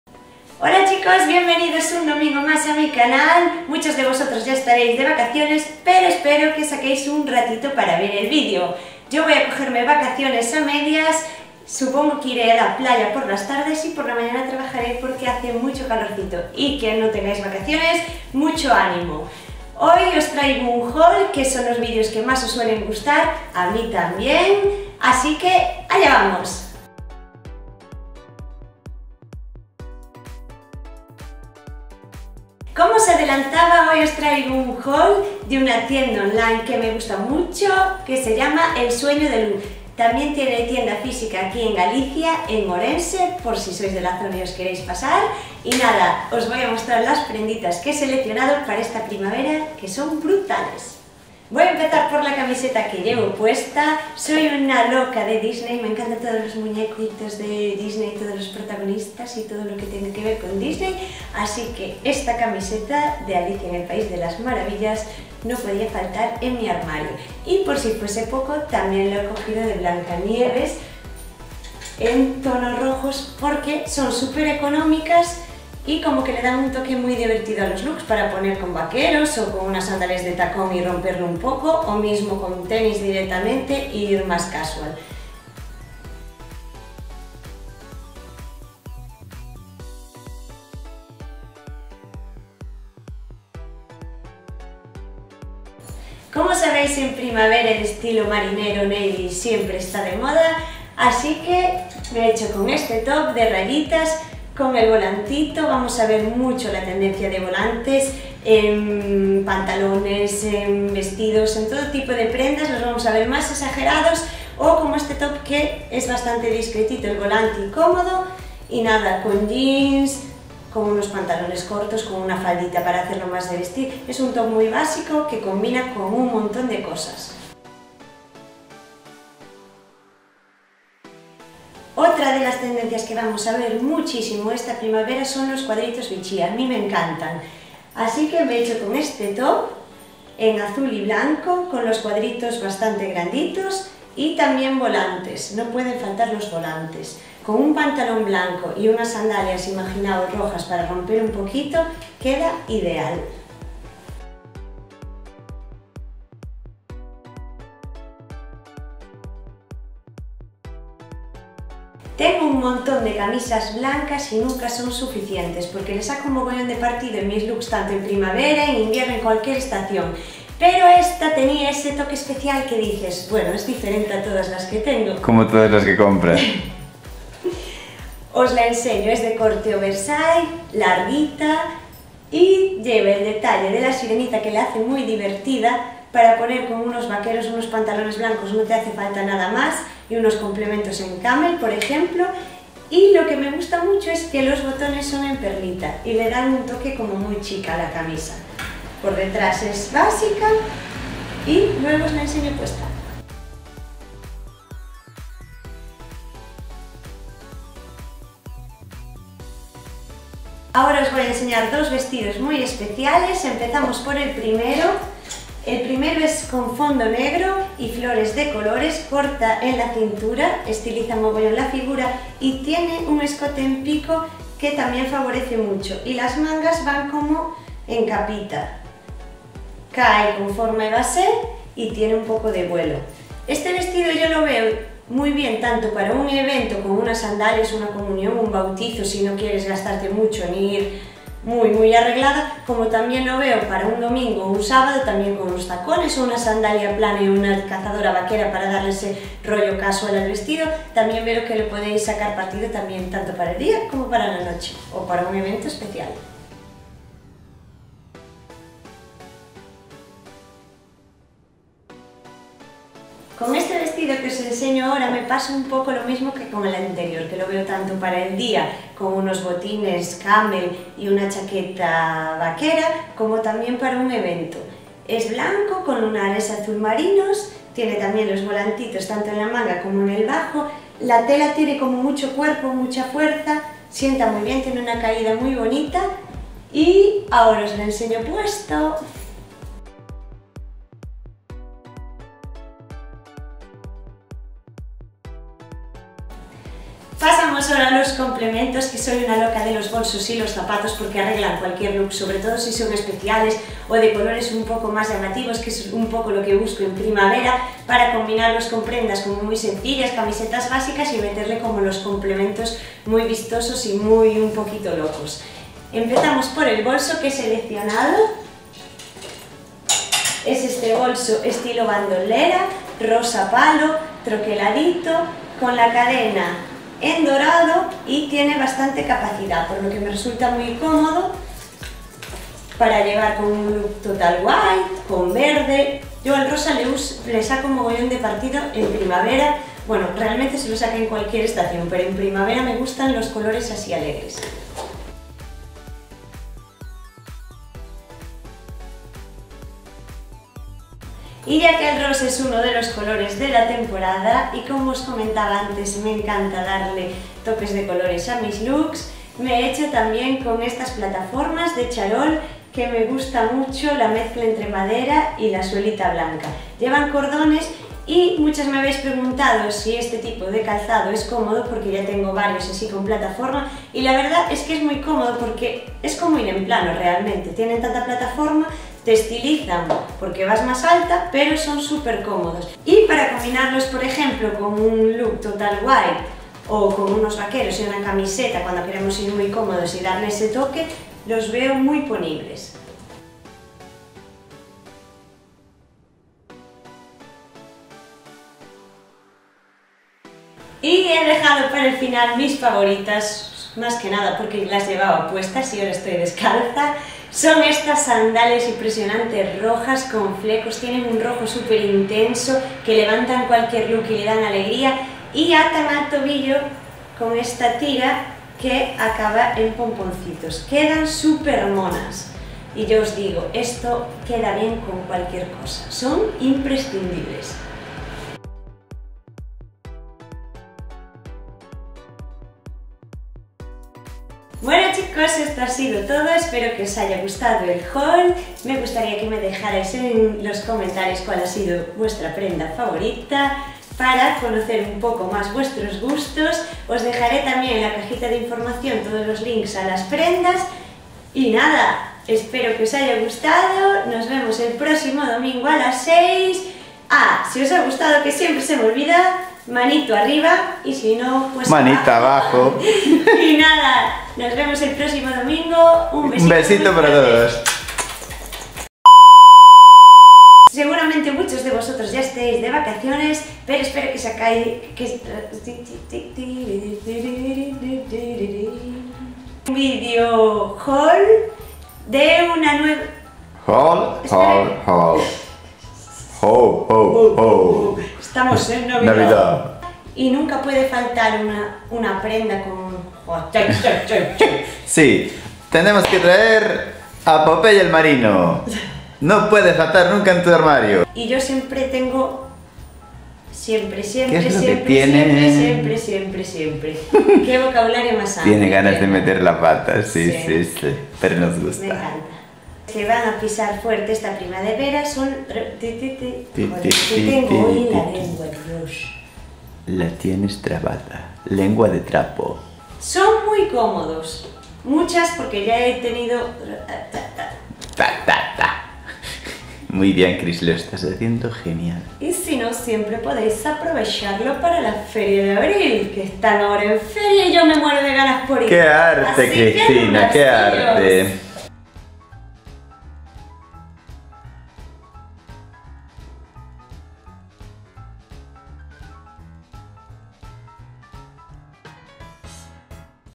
HAUL EL SUEÑO DE LU
Hola amores! Hoy os traigo un haul de una tienda El sueño de lu que me encanta por su variedad de estilos, originalidad y precio. A Continuación os dejo los links a todas las prendas. Espero que os gusten las prendas y el video.
Un besazo fuerte y nos vemos el próximo domingo.